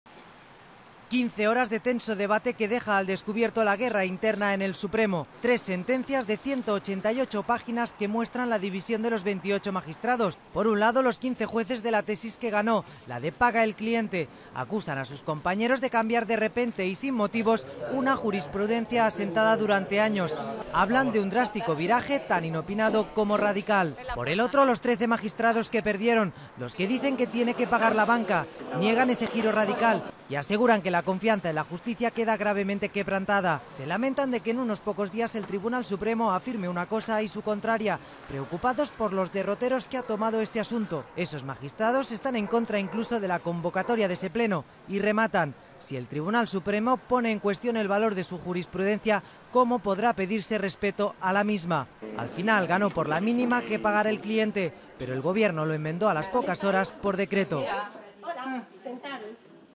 La sentencia del impuesto de las hipotecas provoca una guerra interna en el Supremo
Guerra interna en el Tribunal Supremo. La publicación de la sentencia del impuesto de las hipotecas ha puesto en evidencia las discrepancias entre los magistrados. Once de los jueces incluyen duras críticas al fallo que cargaba la tasa a los clientes. Los 12 jueces del Supremo contrarios a la sentencia hablan de confianza "quebrantada" en la justicia. Los otros 15 magistrados acusan  a sus compañeros de "arbitrarismo".

Fuente: ATLAS